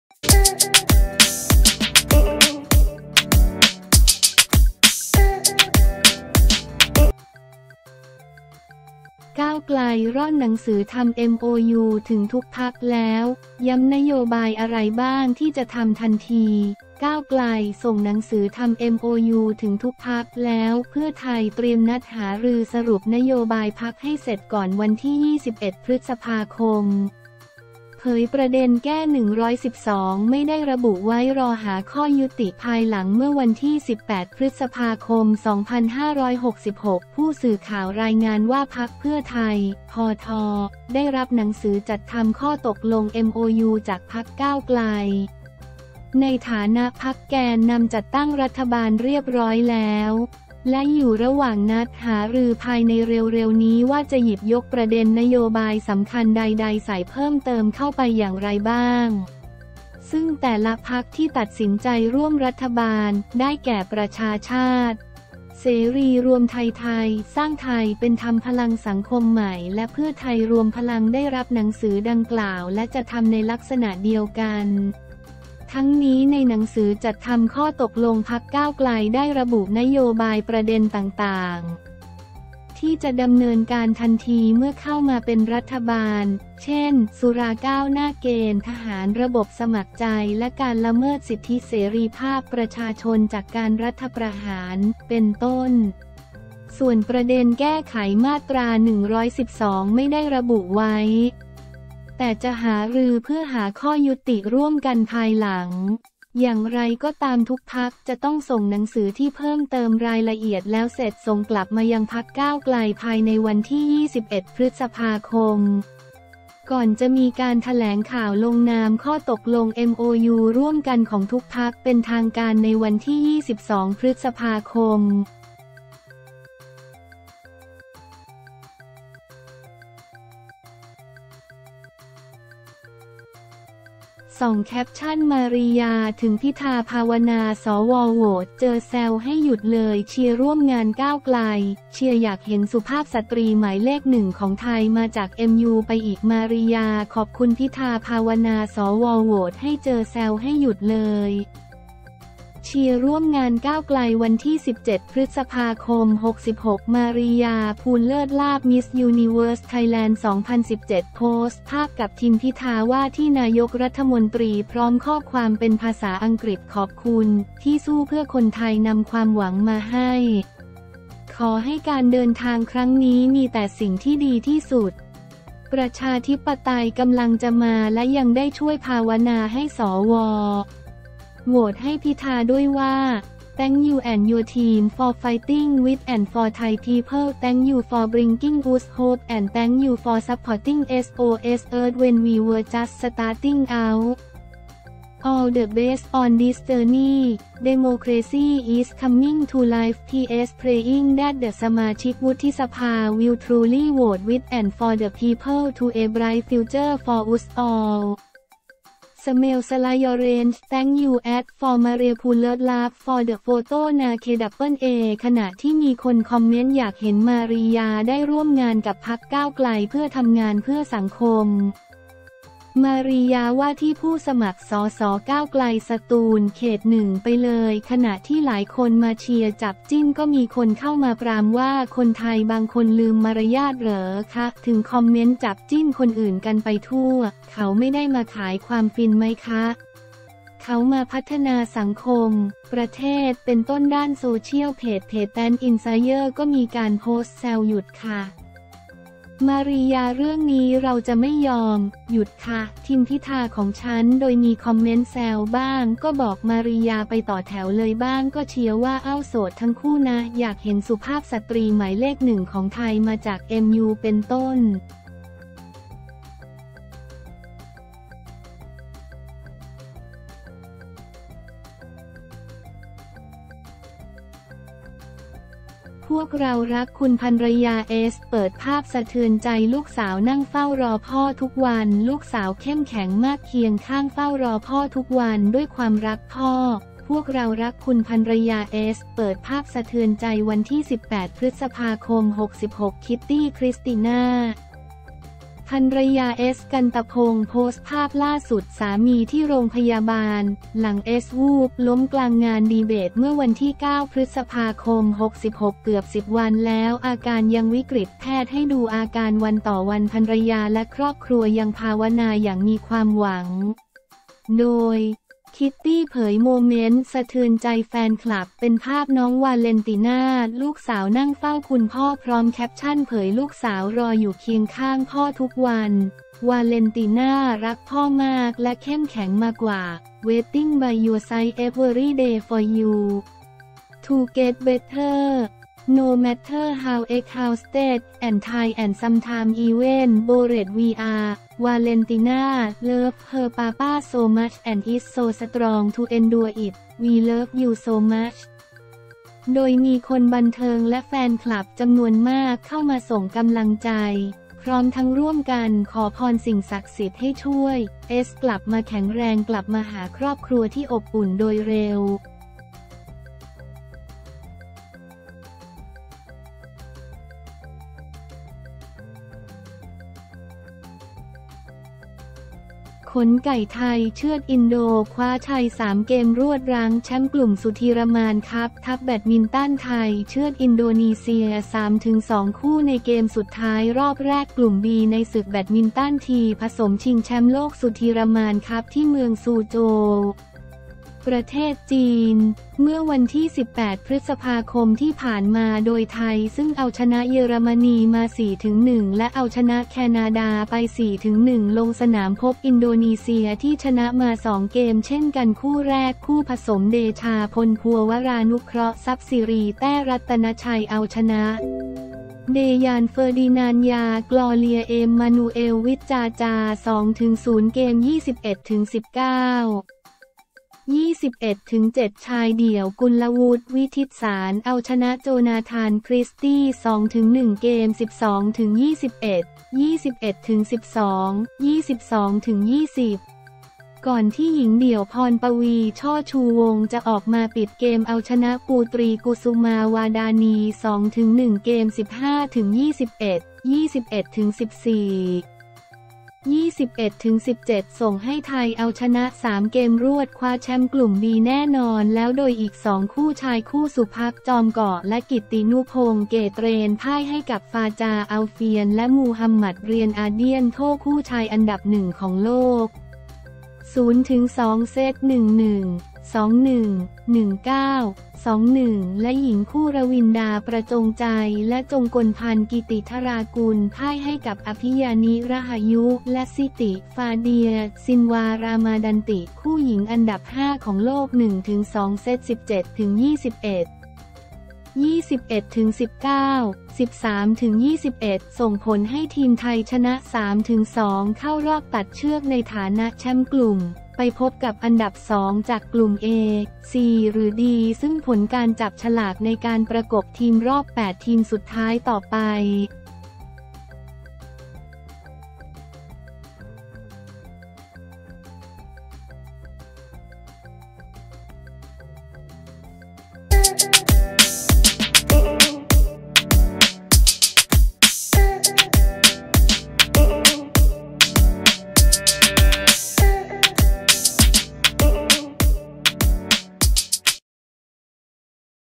ก้าวไกล ร่อนหนังสือทำเอ็มโอยูถึงทุกพรรคแล้ว ย้ำนโยบายอะไรบ้างที่จะทำทันที
ก้าวไกล ส่งหนังสือทำเอ็มโอยู ถึงทุกพรรคแล้ว เพื่อไทย เตรียมนัดหารือสรุปนโยบายพรรคให้เสร็จก่อน 21 พ.ค. เผยประเด็นแก้ 112 ไม่ได้ระบุไว้ รอหาข้อยุติภายหลัง
เมื่อวันที่ 18 พ.ค.2566 ผู้สื่อข่าวรายงานว่า พรรคเพื่อไทย (พท.) ได้รับหนังสือจัดทำข้อตกลง MOU จากพรรคก้าวไกล ในฐานะพรรคแกนนำจัดตั้งรัฐบาลเรียบร้อยแล้ว และอยู่ระหว่างนัดหารือภายในเร็วๆ นี้ ว่าจะหยิบยกประเด็นนโยบายสำคัญใดๆ ใส่เพิ่มเติมเข้าไปอย่างไรบ้าง ซึ่งแต่ละพรรคที่ตัดสินใจร่วมรัฐบาล ได้แก่ ประชาชาติ เสรีรวมไทย ไทยสร้างไทย เป็นธรรม พลังสังคมใหม่ และเพื่อไทรวมพลัง ได้รับหนังสือดังกล่าวและจะทำในลักษณะเดียวกัน

ทั้งนี้ ในหนังสือจัดทำข้อตกลงพรรคก้าวไกลได้ระบุนโยบายประเด็นต่างๆ ที่จะดำเนินการทันทีเมื่อเข้ามาเป็นรัฐบาล เช่น สุราก้าวหน้า เกณฑ์ทหารระบบสมัครใจ และการละเมิดสิทธิเสรีภาพประชาชนจากการรัฐประหาร เป็นต้น ส่วนประเด็นแก้ไขมาตรา 112 ไม่ได้ระบุไว้ แต่จะหารือเพื่อหาข้อยุติร่วมกันภายหลัง

อย่างไรก็ตามทุกพรรคจะต้องส่งหนังสือที่เพิ่มเติมรายละเอียดแล้วเสร็จส่งกลับมายังพรรคก้าวไกลภายในวันที่ 21 พ.ค. ก่อนจะมีการแถลงข่าวลงนามข้อตกลง MOU ร่วมกันของทุกพรรคเป็นทางการในวันที่ 22 พ.ค.